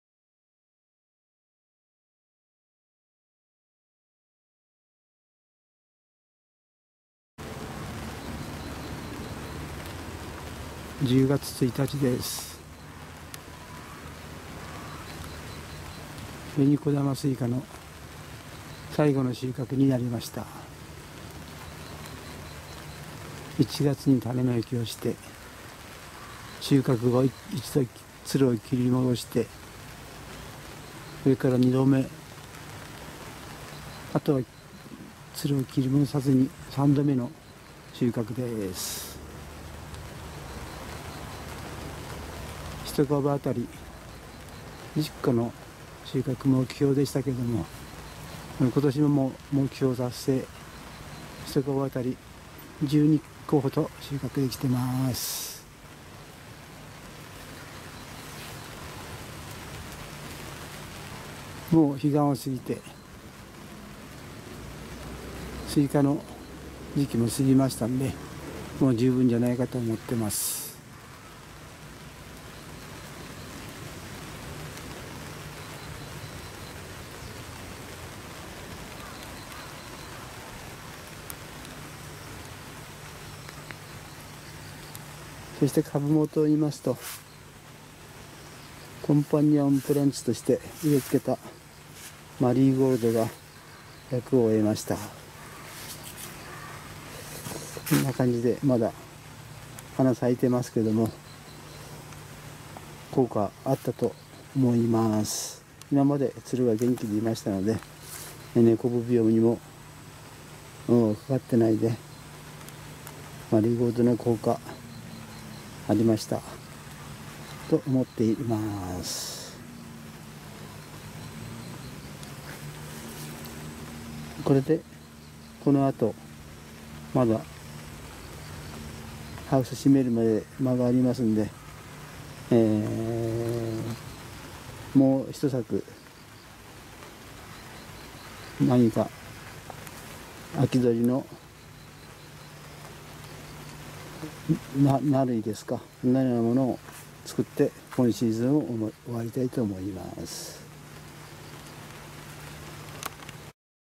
シニアおじさんのスローライフ#126（小玉スイカの栽培を終了する）
１０月１日
紅小玉スイカを収穫して今年のスイカの栽培を終了する